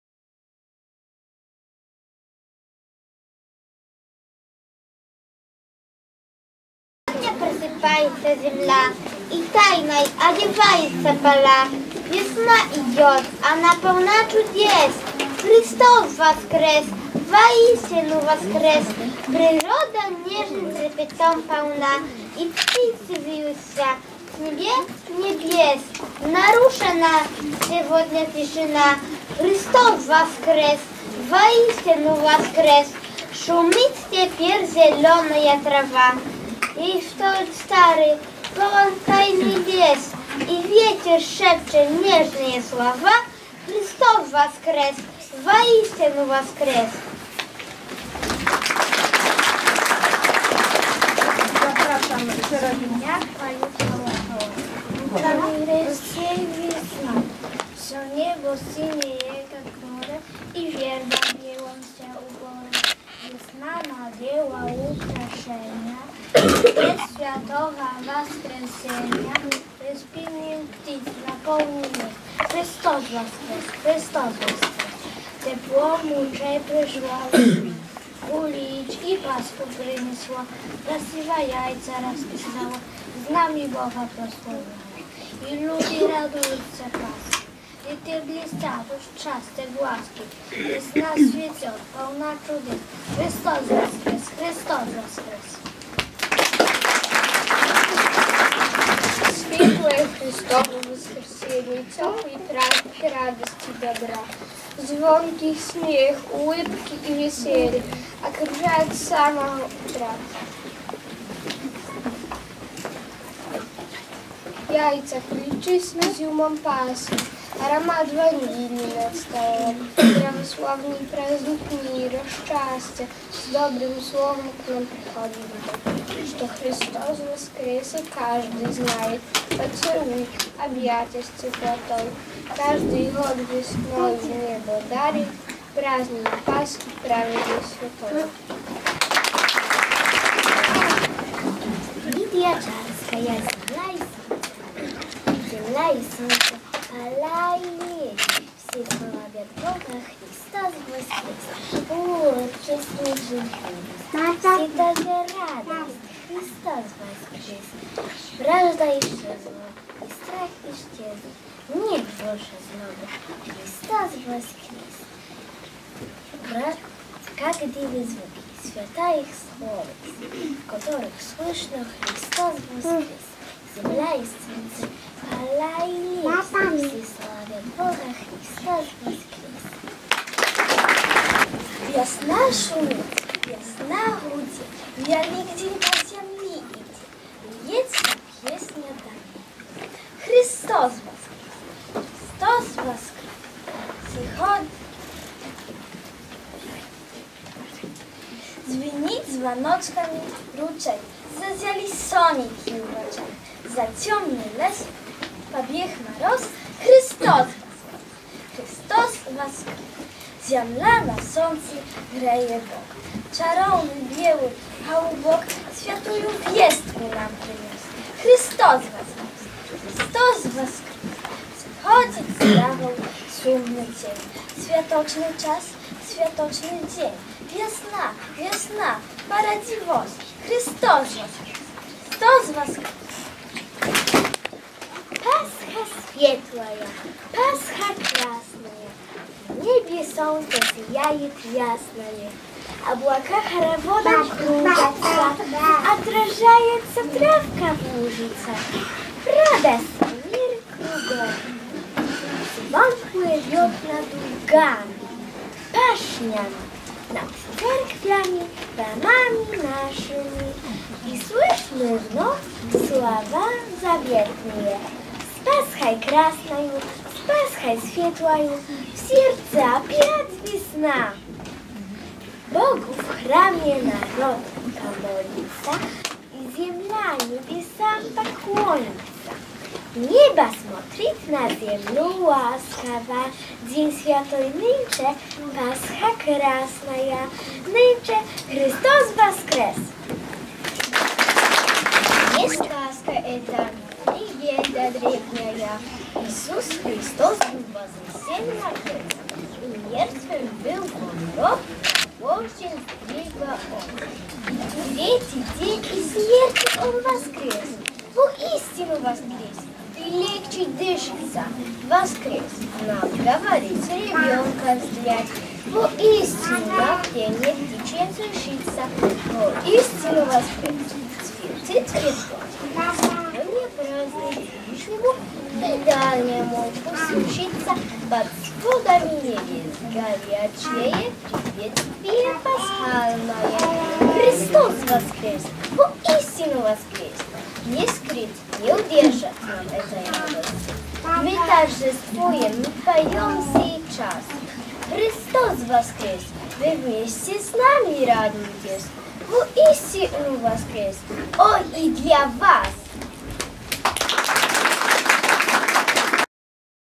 Uczniowie XV Spotkania z Poezją i Prozą Religijną o Tematyce Paschalnej
Bielski Dom Kultury w Bielsku Podlaskim. Recytują uczniowie z Niepublicznej Szkoły Podstawowej nr 1 im.św.Łukasza Wojno-Jasienieckiego.